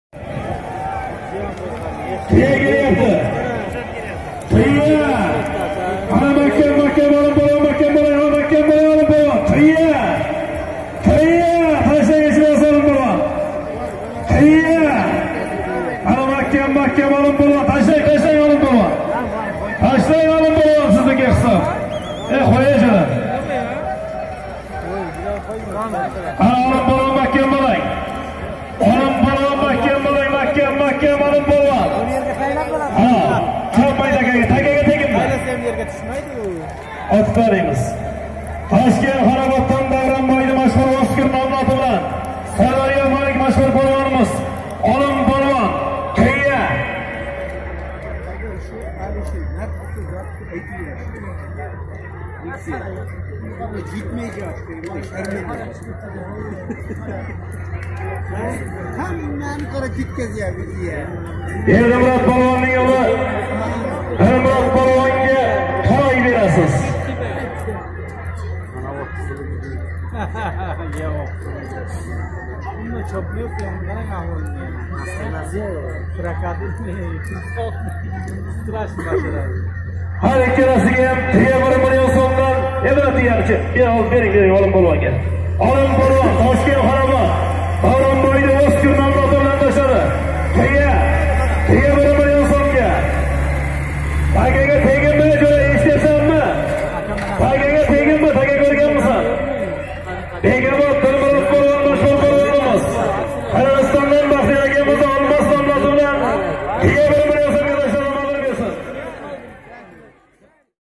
SIRDARYO MALIK OLIM POLVON BAHOM AKANI OSKARIDA SAMARQAND URGUT VAGASHTIDA TUYAGA MARRASI.07.04.2021
3SAMARQAND#kopkari